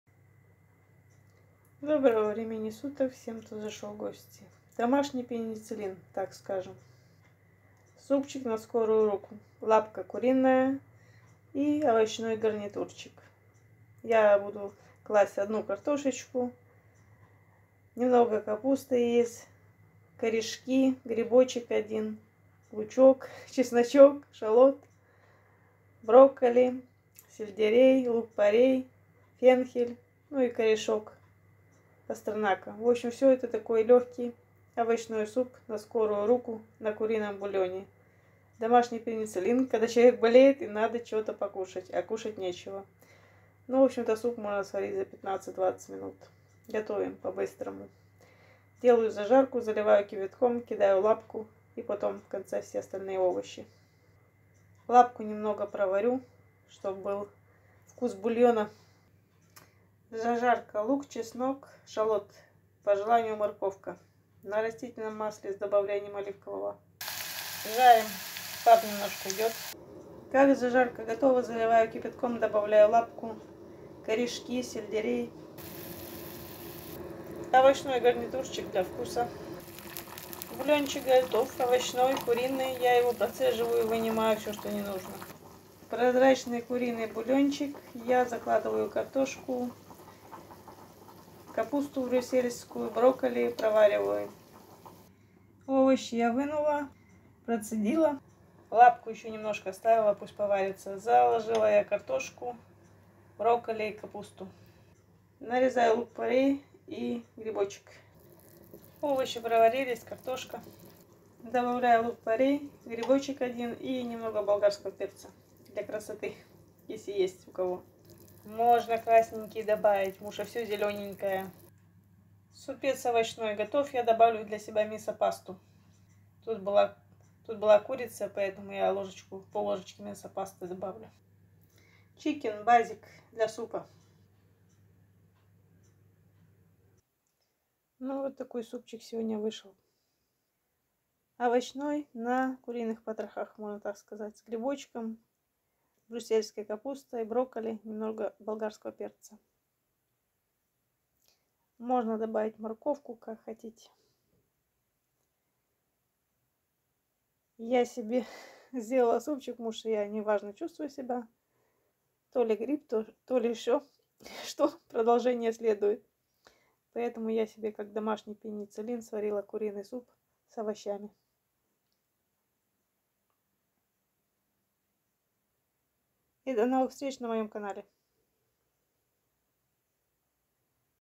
Куриный суп 🍲🍗🧄🥦 с овощами. Мои 150 Супов. Осенне-зимний суп с сезонными овощами/Soupe aux légumes.
Поджарить зажарку из овощей и залить кипятком или бульоном. В бульон кладём куриные потроха (или к зажарке добавьте). Затем картофель, брокколи, брюссельскую капусту, варим до готовности овощей. В конце добавить грибы, лук порей, специи, зелень.